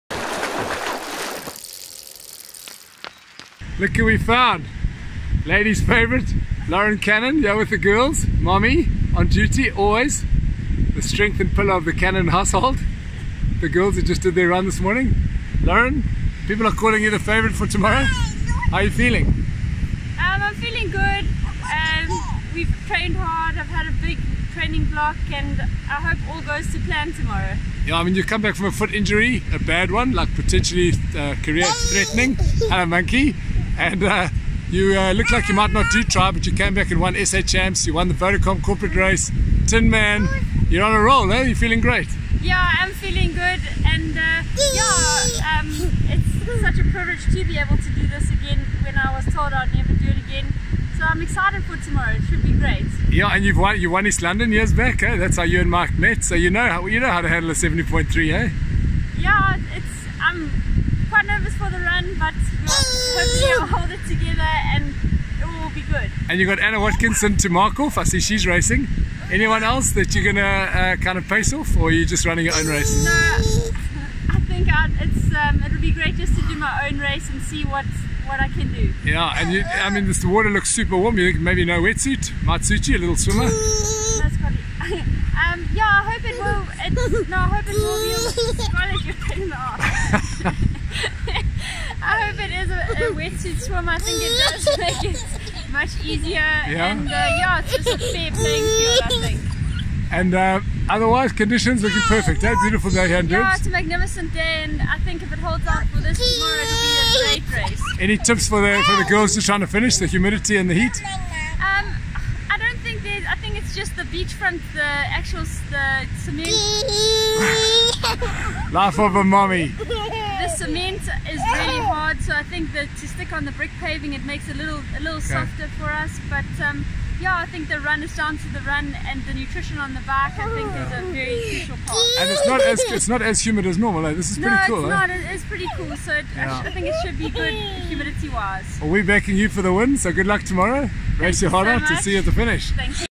Lauren Cannon Pre IRONMAN Durban 70.3 2019
The working mom of two who was almost forced out of tri by a serious injury has had an amazing year since her foot op a year ago. She capped it with a dominant wire to wire win yesterday.

We caught up with Lauren Cannon on babysitting duty on Saturday while husband Mike was doing a warmup swim.